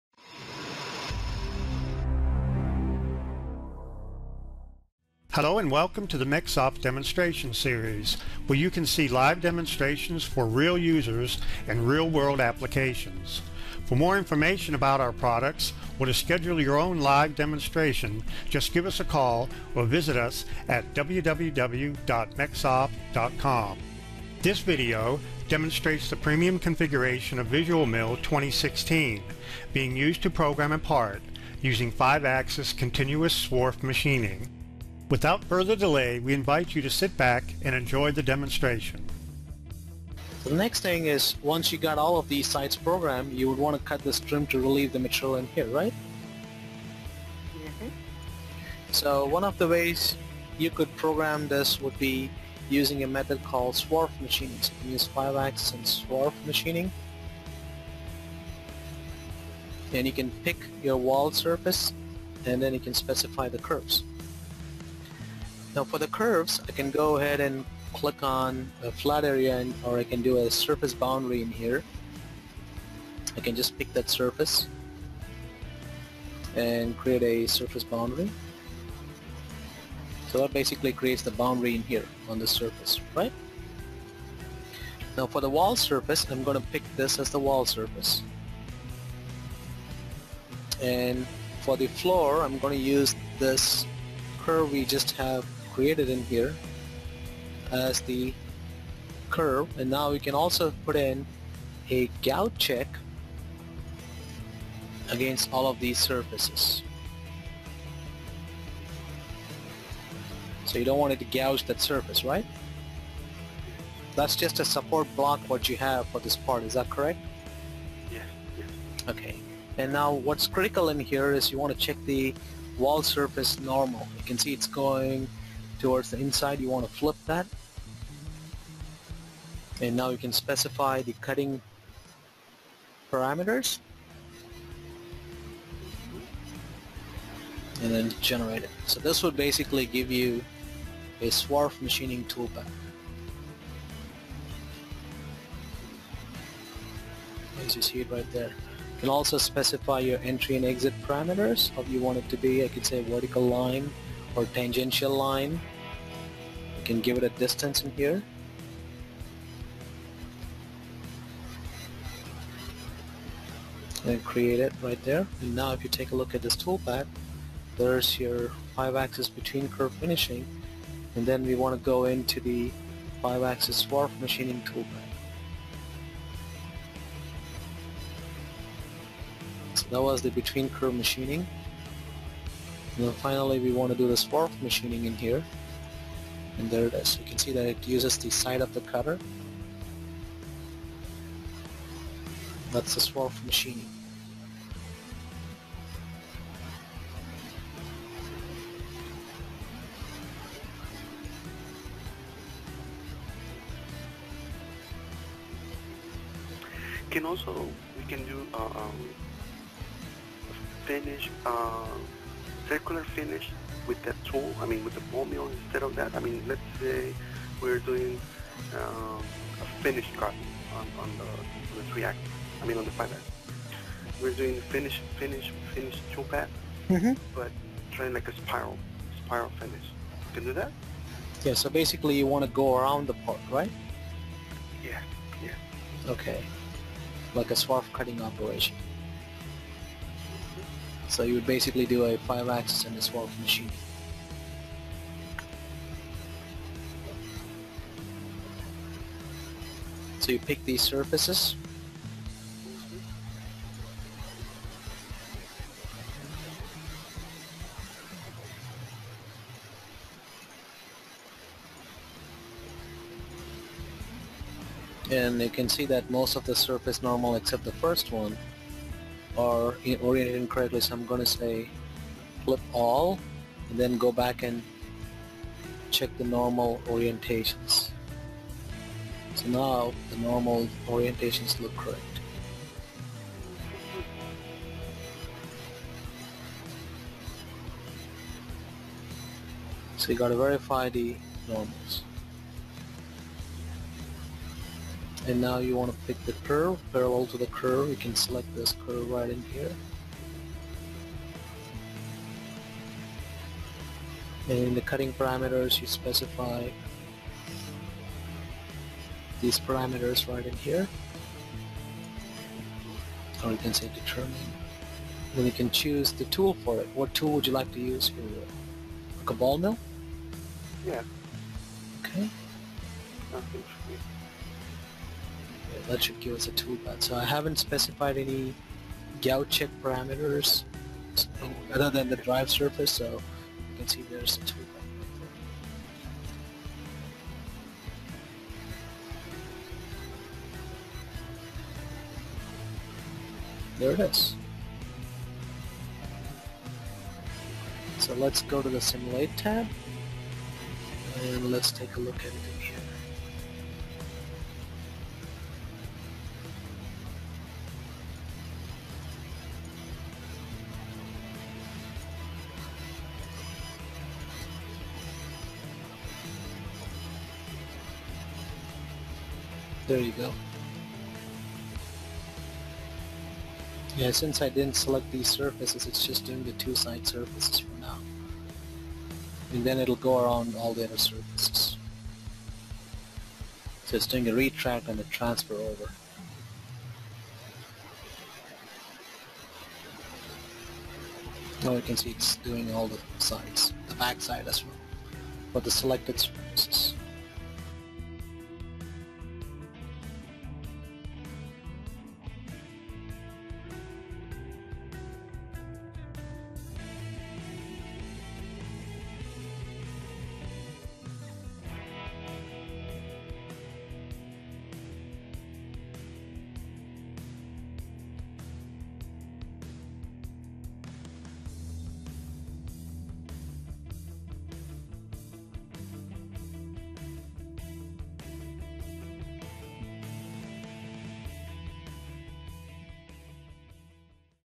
VisualMILL 2016 Continous 5 Axis Swarf Machining
Customer demo of Continuous 5 Axis Swarf Machining using VisualMILL 2016